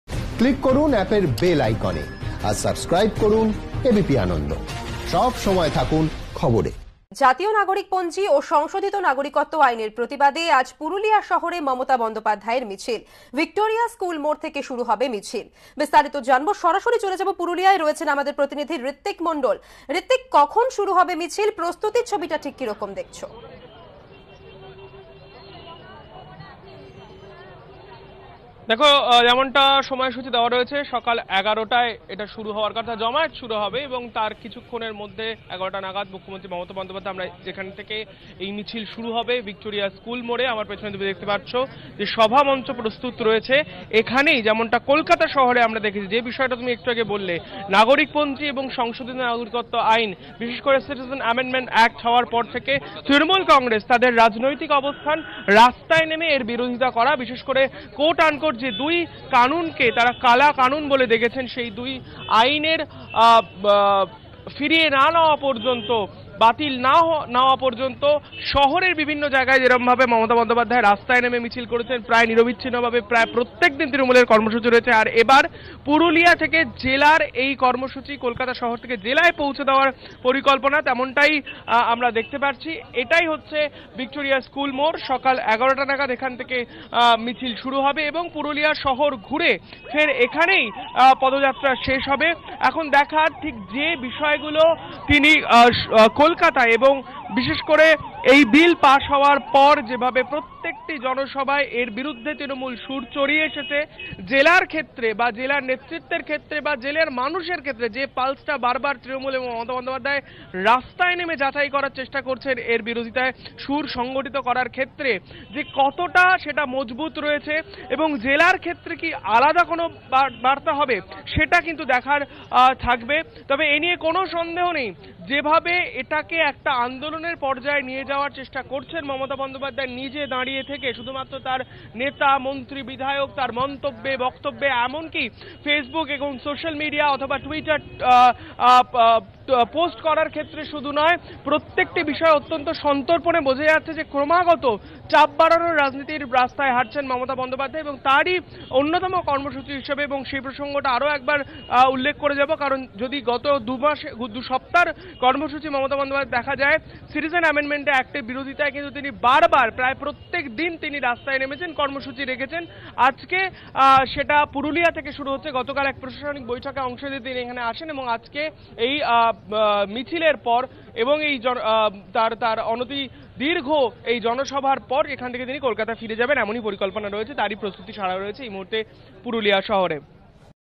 পুরুলিয়ায় মমতার মিছিল, পাল্টা কোচবিহারে অভিনন্দন যাত্রা বিজেপির | ABP Ananda
জাতীয় নাগরিক পঞ্জি ও সংশোধিত নাগরিকত্ব আইনের প্রতিবাদে আজ পুরুলিয়া শহরে মমতা বন্দ্যোপাধ্যায়ের মিছিল।  আইনের সমর্থনে কোচবিহারে বিজেপির অভিনন্দন যাত্রা।